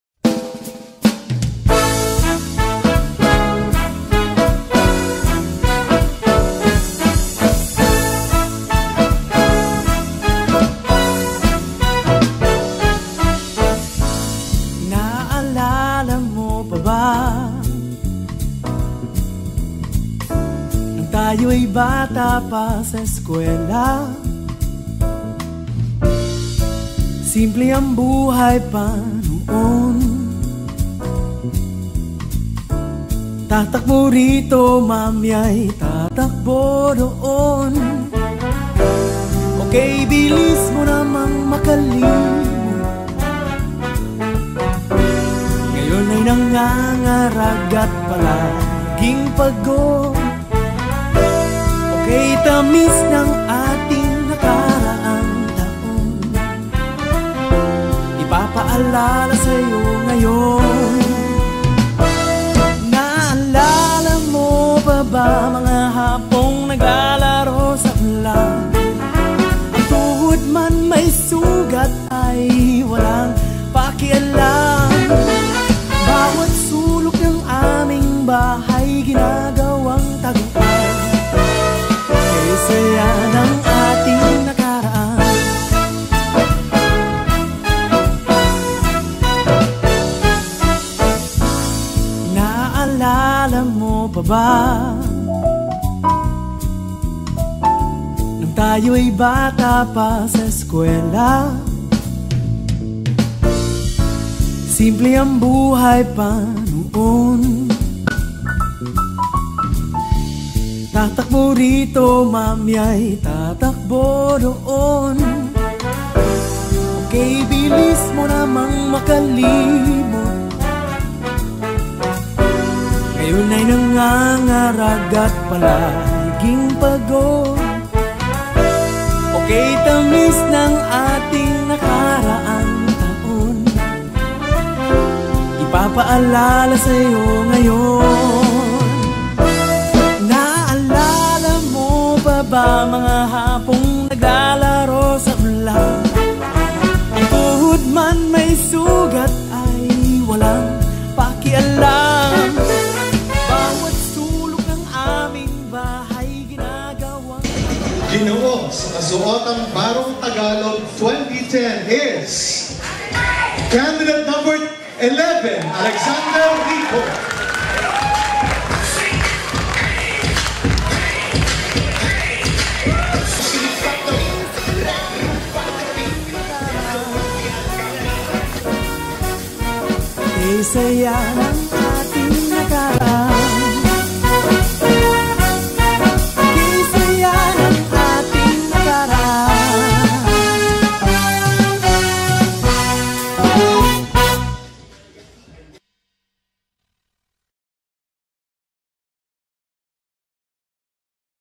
GINOONG FILIPINAS 2010 - Barong Tagalog Competition
You've seen the boys in their barest minimum... this time, get to see them fully and elegantly-clothed, all wholesome and proper - the kind that girls would love to bring home to meet Mom and Dad, so to speak :) ... here are the contestants of the 2010 GINOONG FILIPINAS wearing the traditional Barong Tagalog.